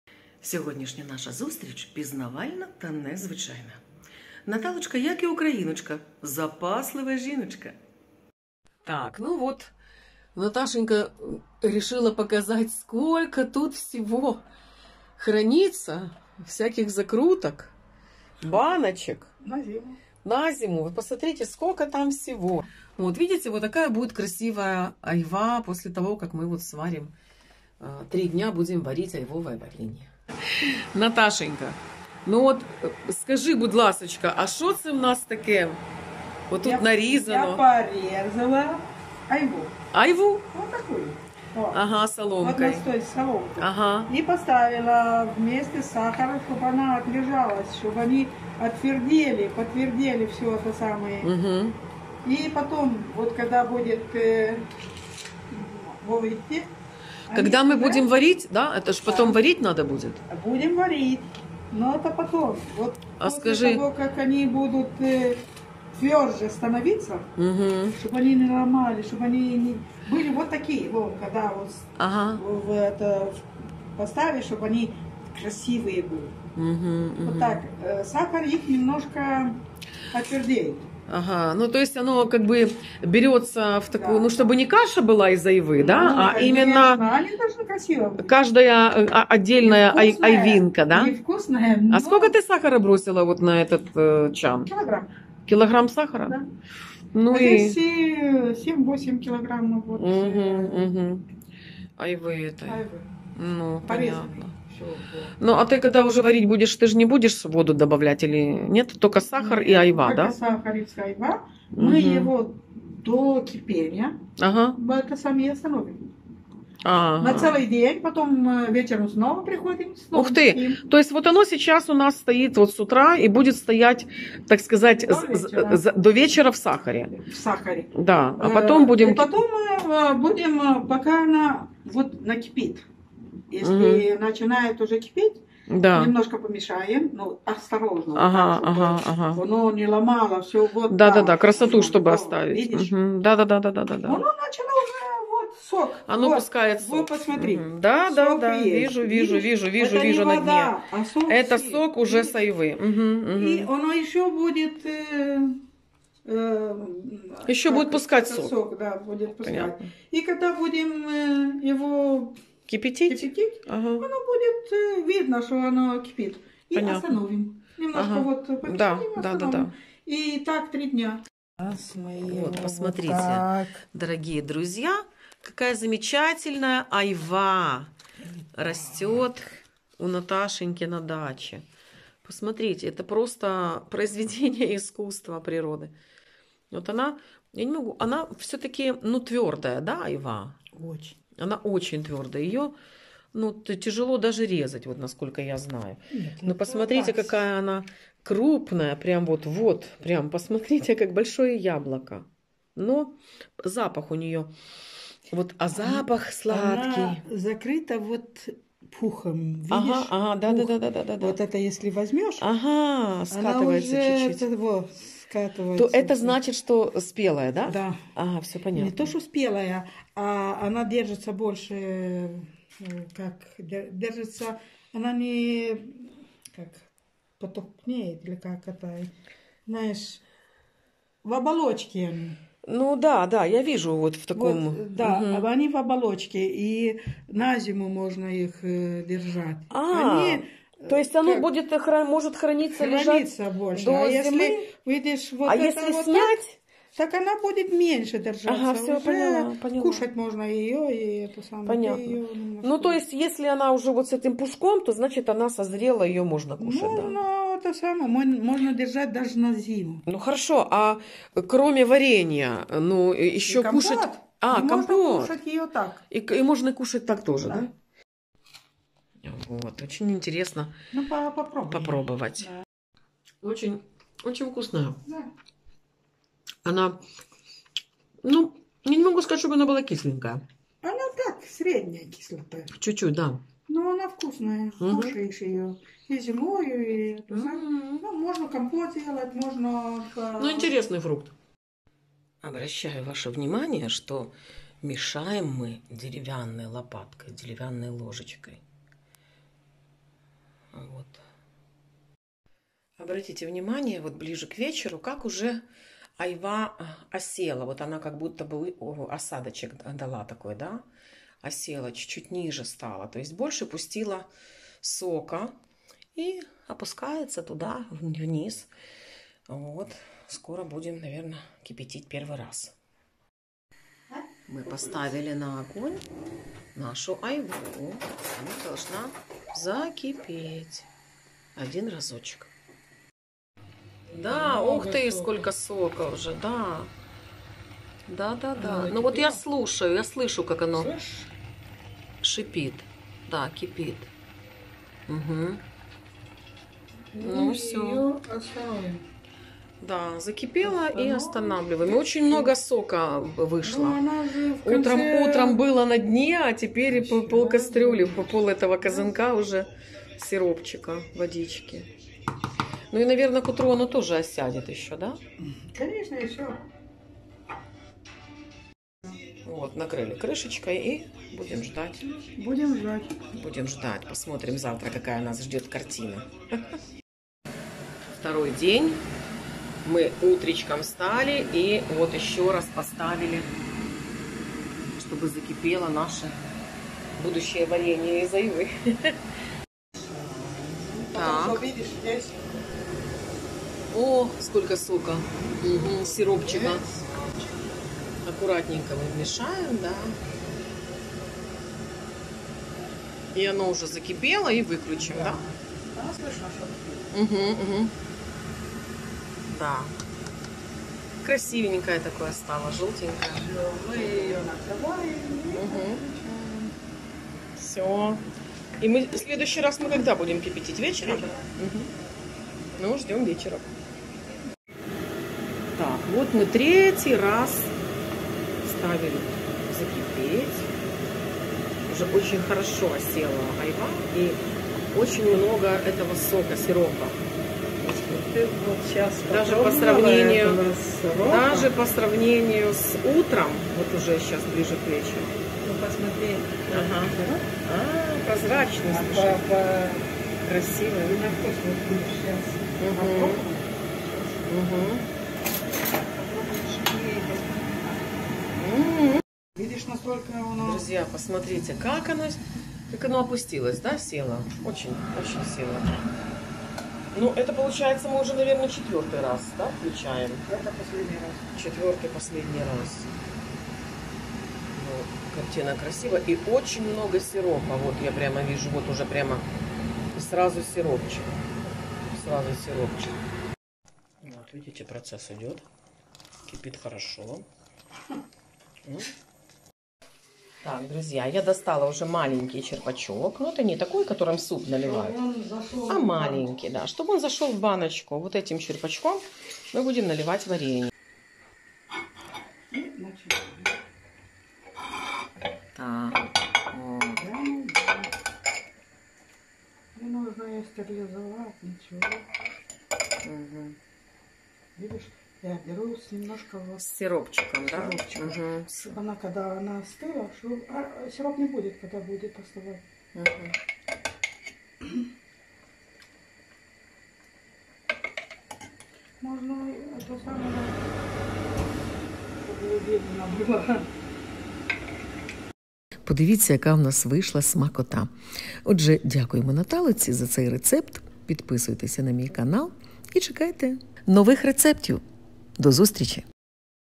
Варення з айви!
Айва – плід, який заслуговує на особливу увагу, адже він не тільки смачний, а й корисний. Ми підготували для вас  рецепт варення на зиму з айви. Смачного!